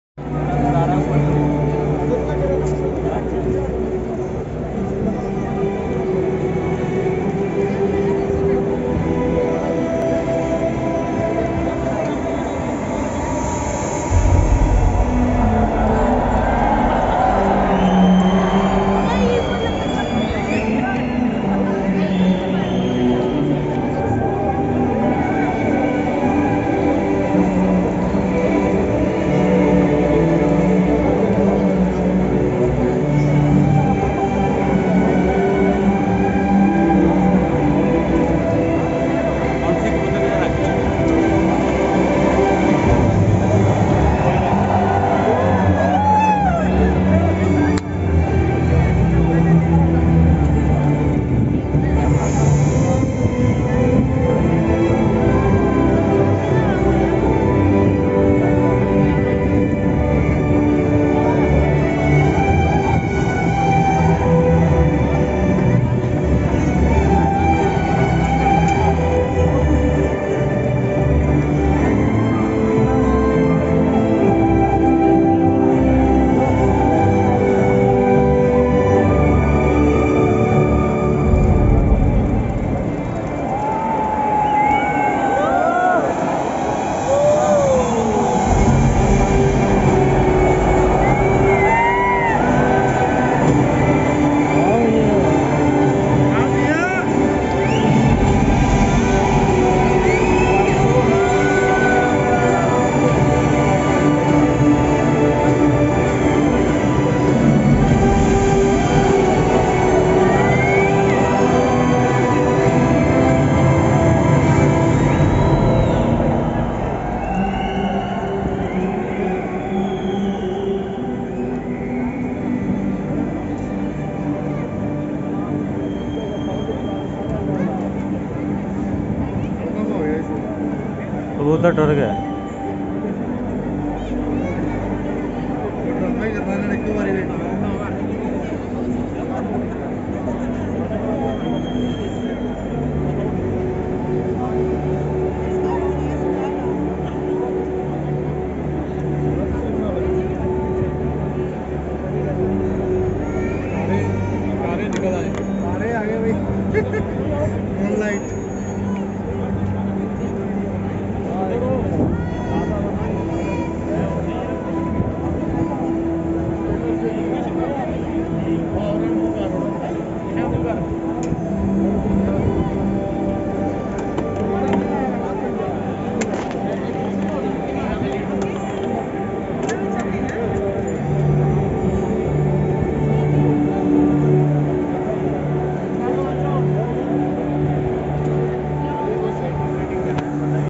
Expo 2020 🤩🤩🔥🔥🔥🔥
A journey that reflects the character of this unique and extraordinary city. the story of Expo 2020 Dubai and how we turned a dream into a reality